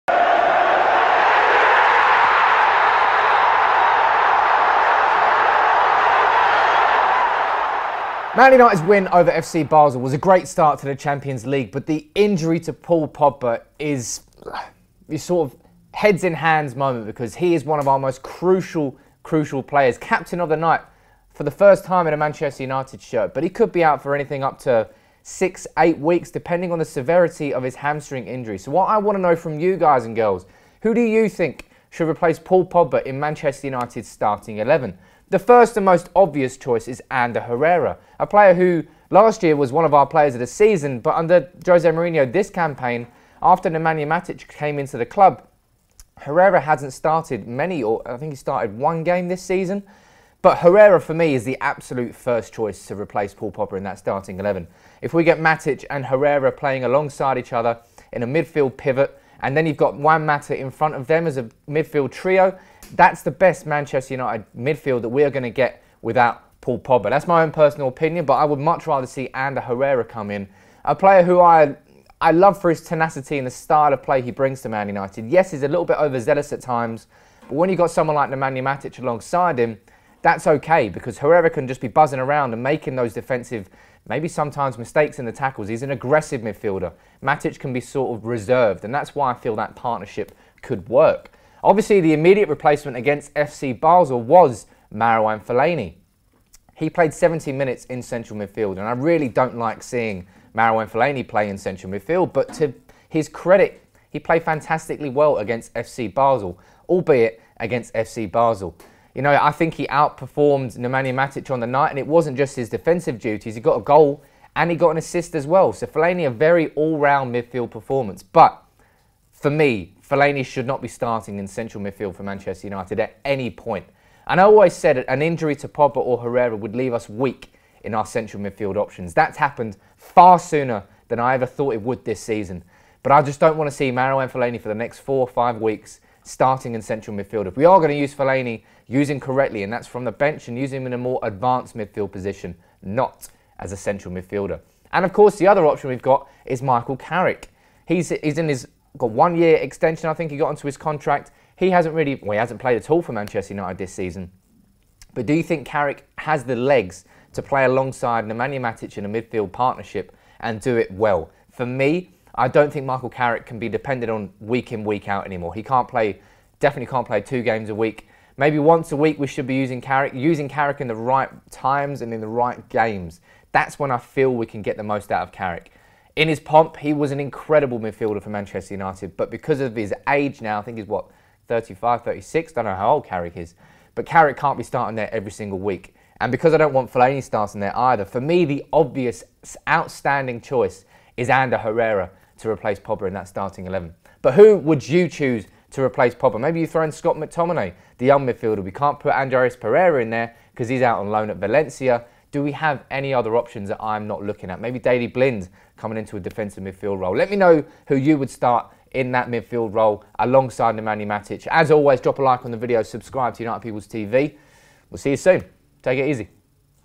WHO SHOULD REPLACE PAUL POGBA? | FELLAINI, HERRERA OR CARRICK?
Paul Pogba is sidelined with a hamstring injury after FC Basel, so who should replace him for Manchester United?